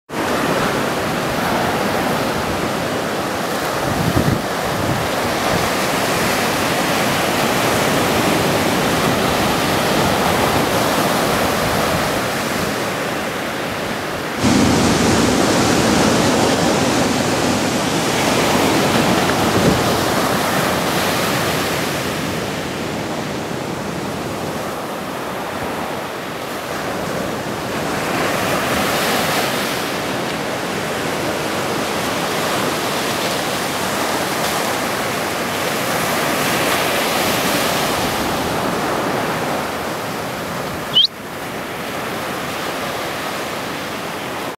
Heavy Wedge Wipeouts
Heavy wipeouts at the Wedge, Newport Beach, CA.  Video of the Wedge doing evil things to bodyboarders, bodysurfers and surfers ...